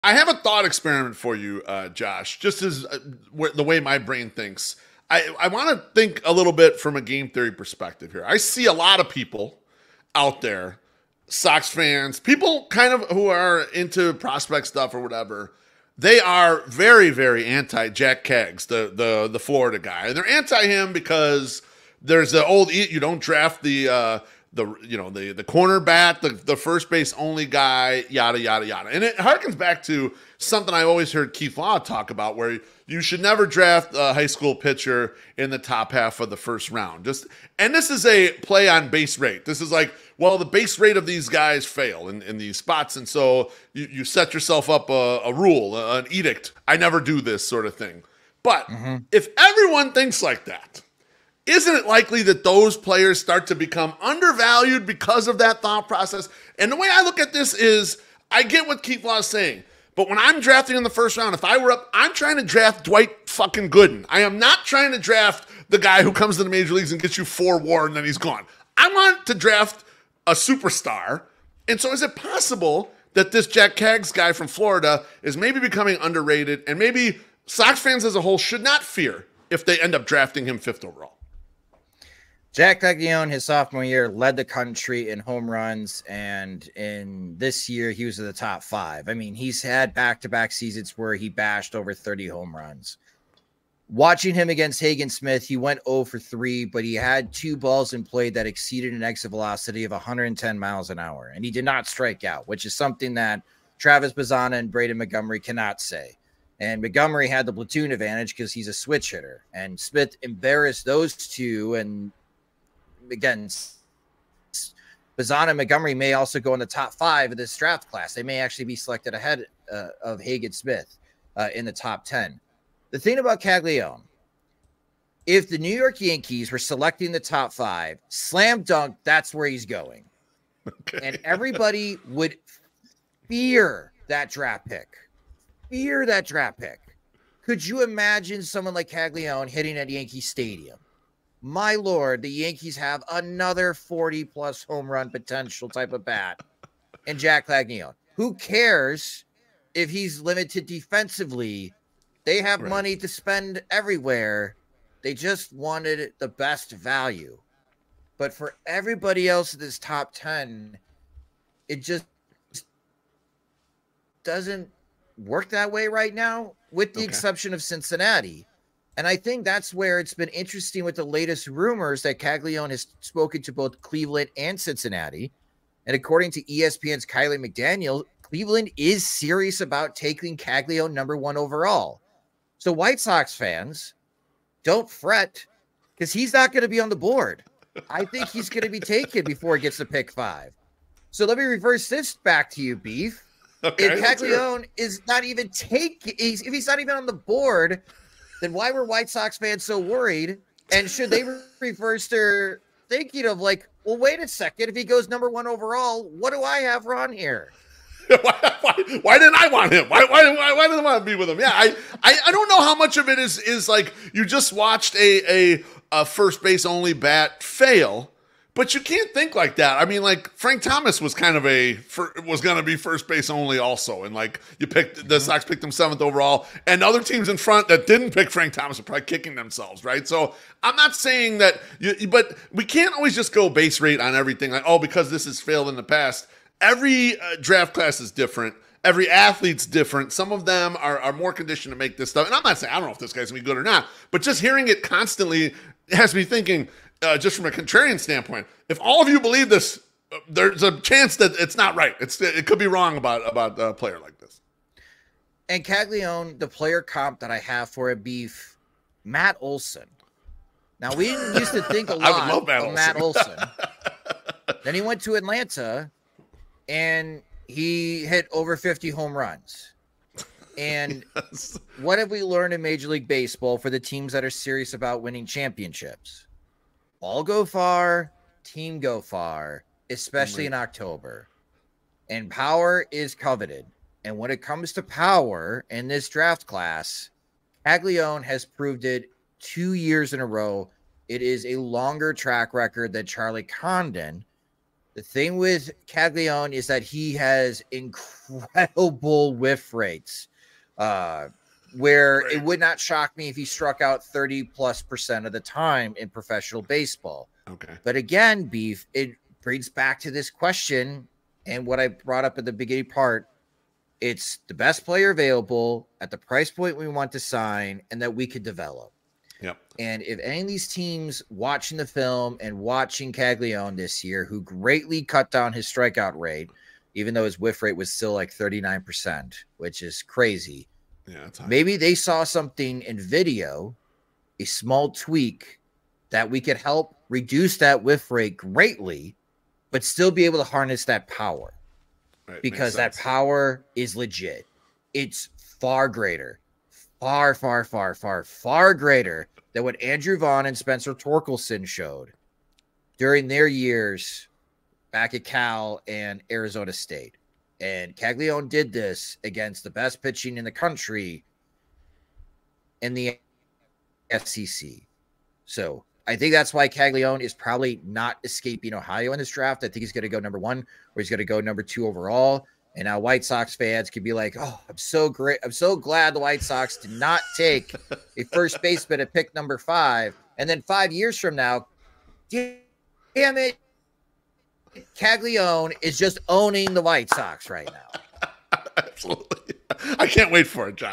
Why are White Sox fans afraid of Jac Caglianone?
Josh Nelson joins The Au Jus and we discuss the recent fan backlash of the Florida 1st baseman in the 2024 MLB Draft